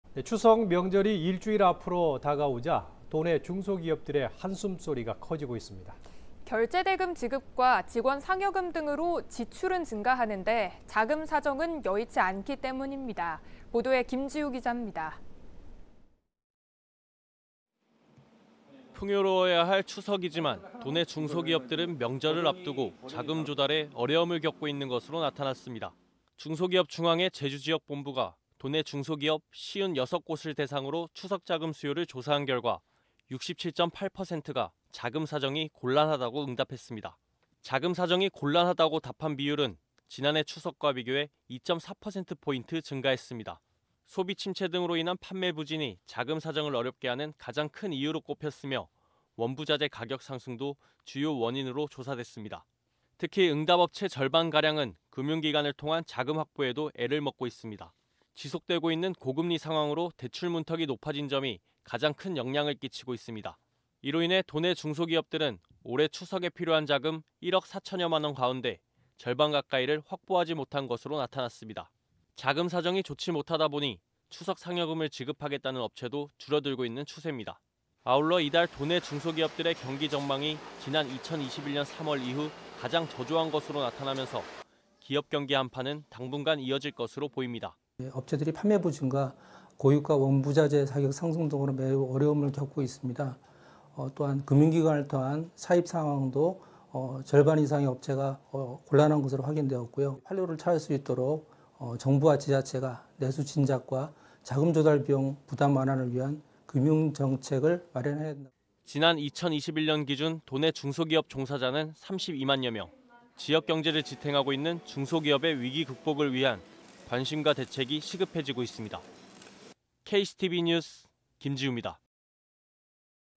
"자금 사정 어렵다"…반갑지만은 않은 추석
추석 명절이 일주일 앞으로 다가오면서 도내 중소기업들의 한숨이 커지고 있습니다.

결제 대금 지급과 직원 상여금 등으로 지출은 증가하는데 자금 사정은 여의치 않기 때문입니다.